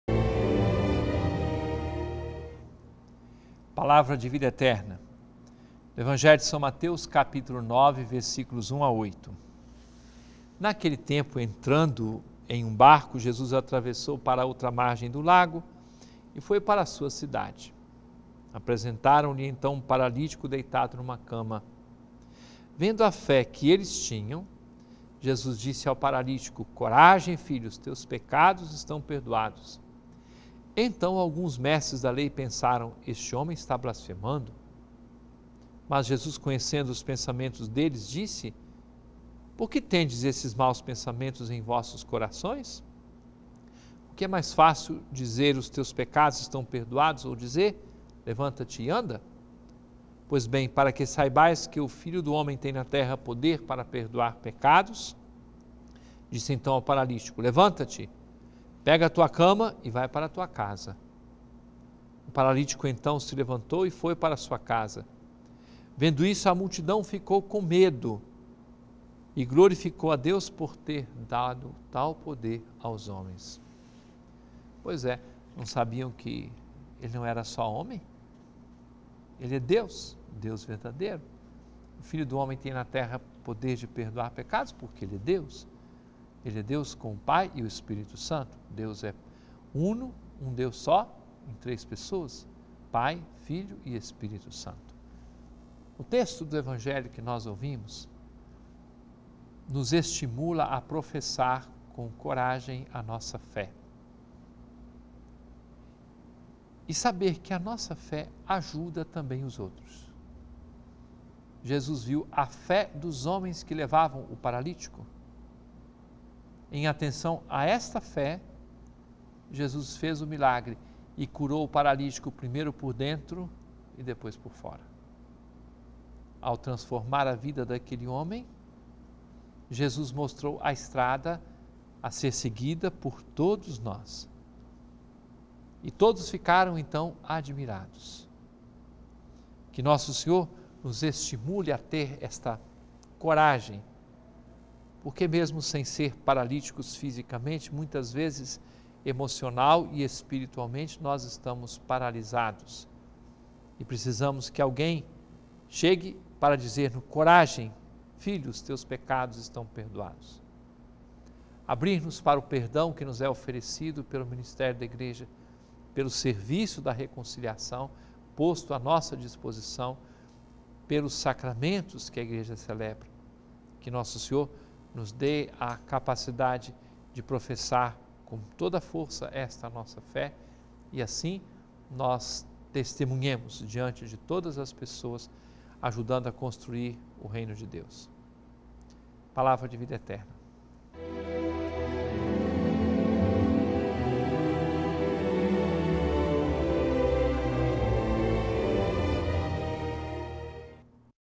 Mateus 9,1-8 - 05/07/2012
Naquele tempo, 1entrando em um barco, Jesus atravessou para a outra margem do lago e foi para a sua cidade. 2Apresentaram-lhe, então, um paralítico deitado numa cama. Vendo a fé que eles tinham, Jesus disse ao paralítico: "Coragem, filho, os teus pecados estão perdoados!" 3Então alguns mestres da Lei pensaram: "Esse homem está blasfemando!" 4Mas Jesus, conhecendo os pensamentos deles, disse: "Por que tendes esses maus pensamentos em vossos corações? 5O que é mais fácil, dizer: 'Os teus pecados estão perdoados', ou dizer: 'Levanta-te e anda'? 6Pois bem, para que saibais que o Filho do Homem tem na terra poder para perdoar pecados, — disse, então, ao paralítico — "Levanta-te, pega a tua cama e vai para a tua casa". 7O paralítico então se levantou, e foi para a sua casa. 8Vendo isso, a multidão ficou com medo e glorificou a Deus, por ter dado tal poder aos homens.